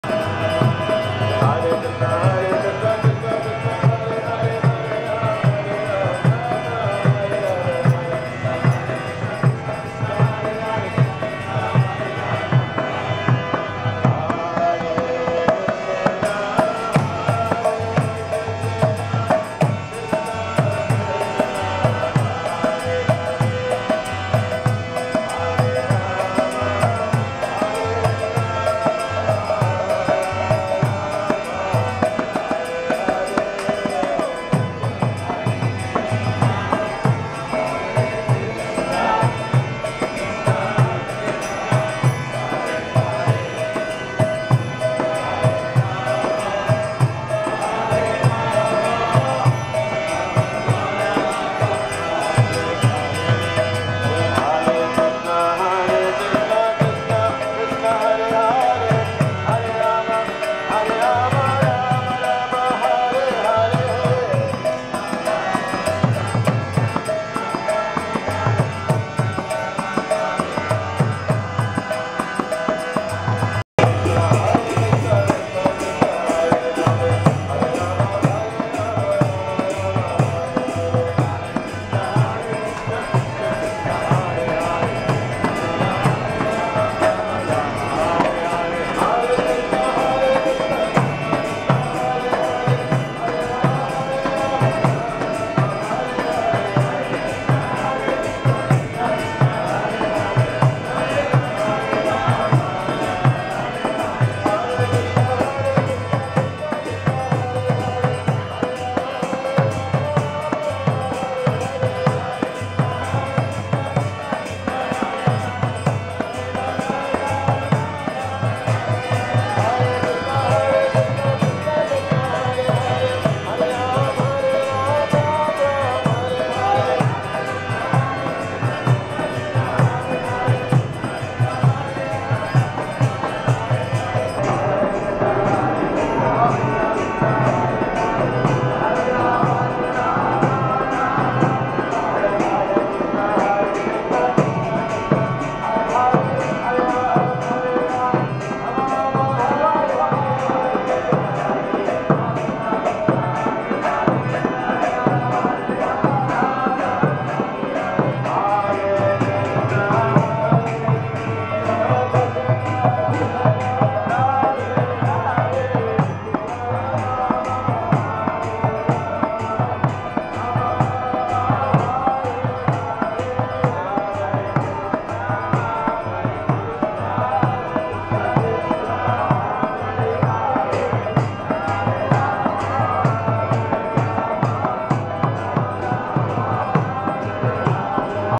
Kaliya Krishna Prabhu Chants and Devotees Dance in Jackson Heights
Kaliya Krishna Prabhu chants and devotees dance in Jackson Heights / Roosevelt Avenue subway station. November 18, 2016.